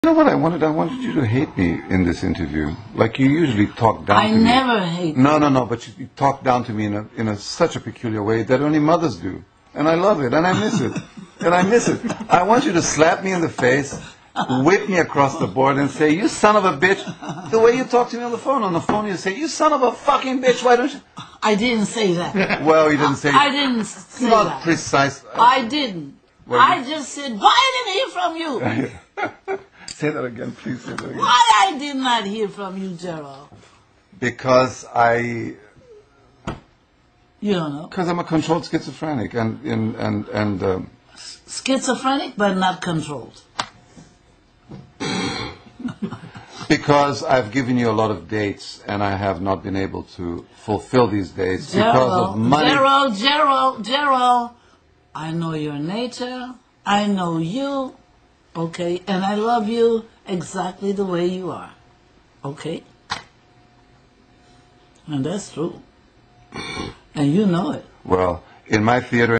ELLEN STEWART AND GERALD THOMAS "I WANT YOU TO SLAP ME"
ELLEN STEWART (La MaMa ETC) and GERALD THOMAS and their perfect mother-son relationship.